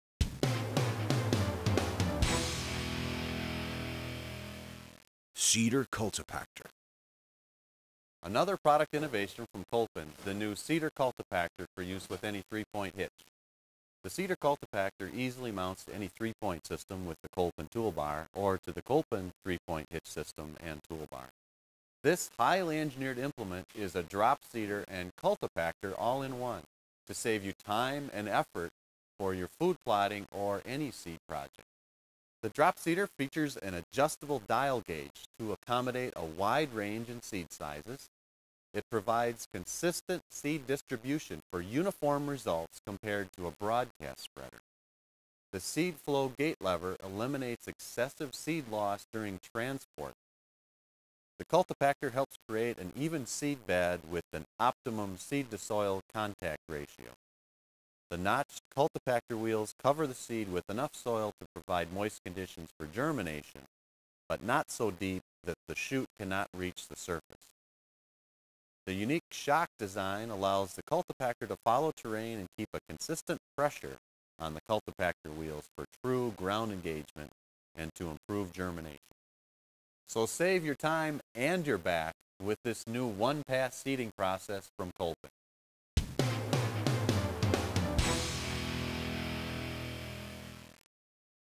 Kolpin Seeder Cultipactor
For use with any 3 point hitch the Kolpin Seeder/Cultipactor is a drop seeder and cultipactor all in one. Used behind your ATV, UTV or existing 3 point hitch it will save you time and effort in your food plotting or any seed project.